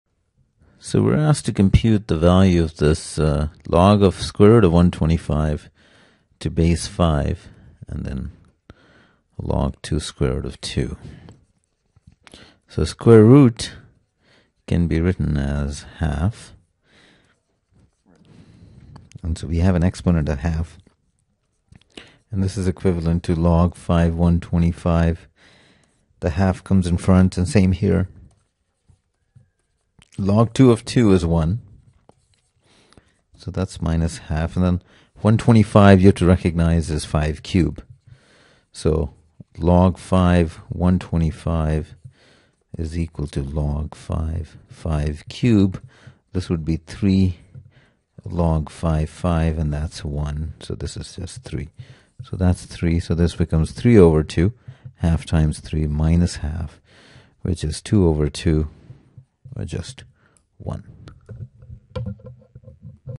Q55 College Algebra 2015 CLEP Official Study Guide
CLEP College Algebra: Video explanations to all of the practice questions in the 2015 Official CLEP Study Guide. Link to book on Amazon